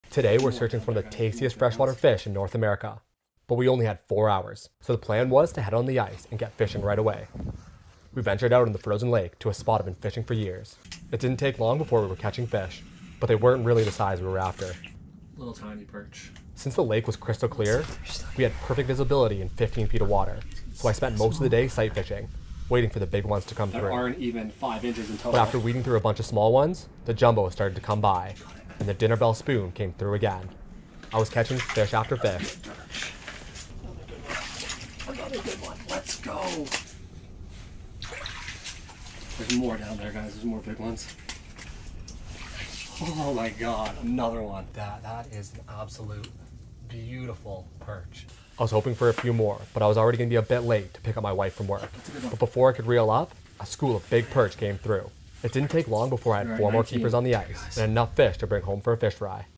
The tastiest freshwater fish around⁉️
We decided to fish one of my favourite lakes to go to in the winter but I only had a few hours to get it done before I had to pick up my wife from work…

I had two rods rigged up one with a pink/silver dinnerbell spoon and another rod with a simple octopus hook and minnow. The spoon ended up catching the biggest fish of the day.

Yellow perch are one of the best fish to target for kids and beginner anglers (not only on the ice but open water as well) They can usually be found in large numbers, they can provide lots of action and are amazing to bring home for a catch and cook.

Can’t wait for my next ice fishing adventure